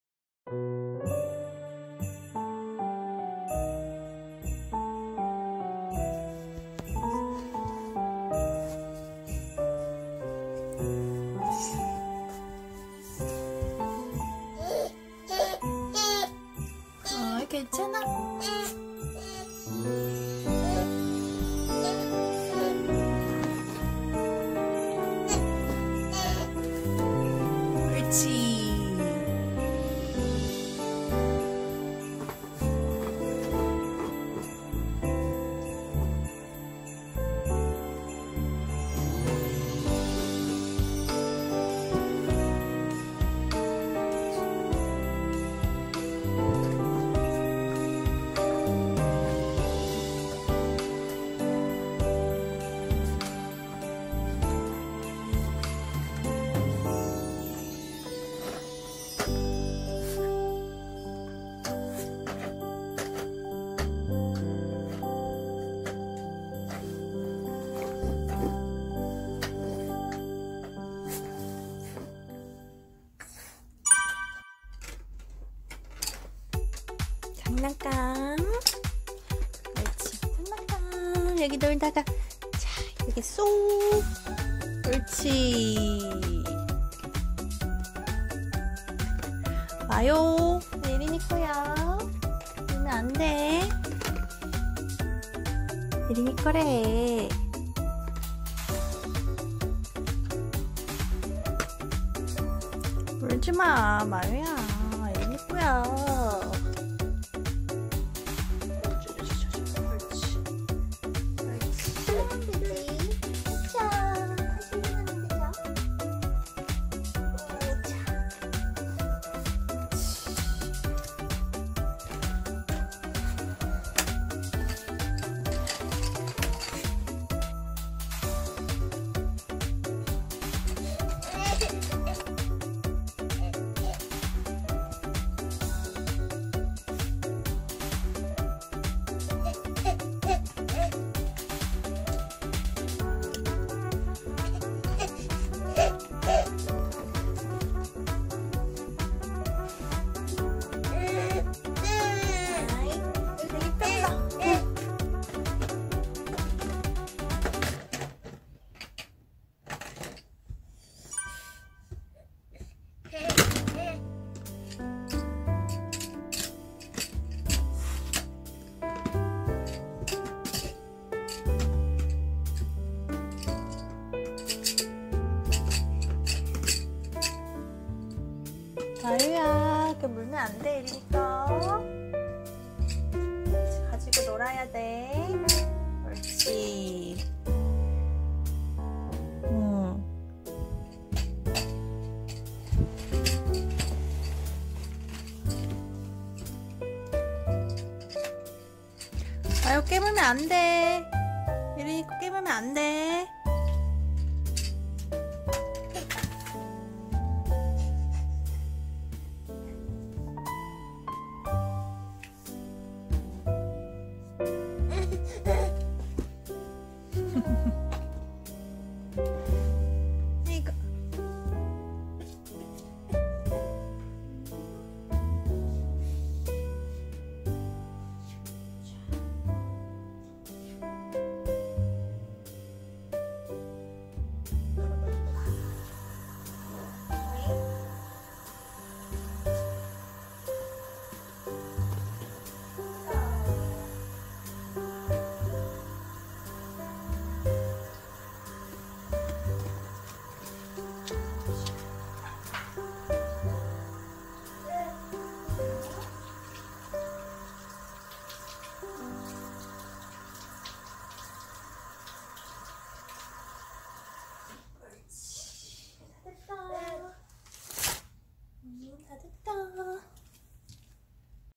(감동) 마요가 예린이를 위로하는 방법 | 화이트테리어 VLOG West Highland White Terrier
MAYO ♀
WEST HIGHLAND WHITE TERRIER
(Born in April 2, 2019)